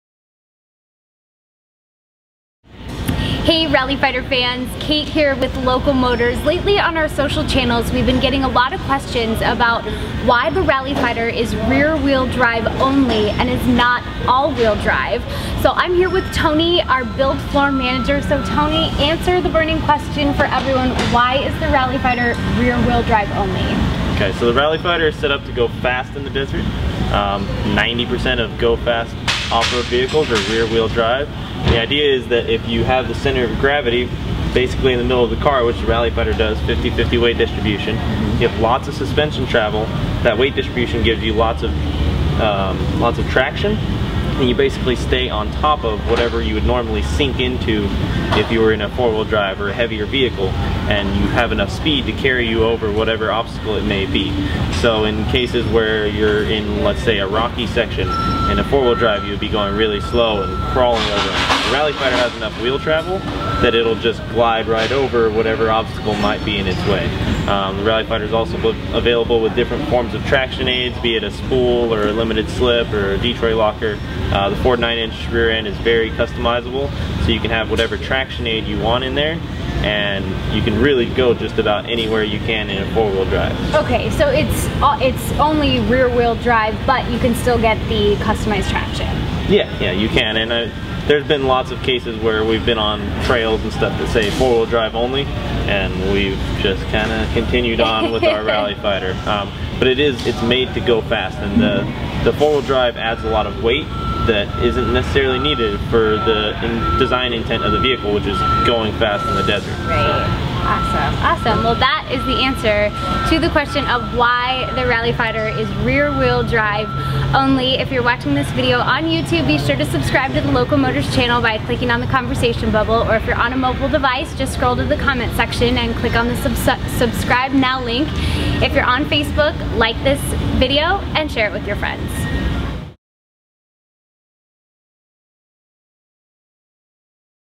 Why the Rally Fighter is RWD
Why is the Rally Fighter rear wheel drive? The micro-factory build floor manager explains.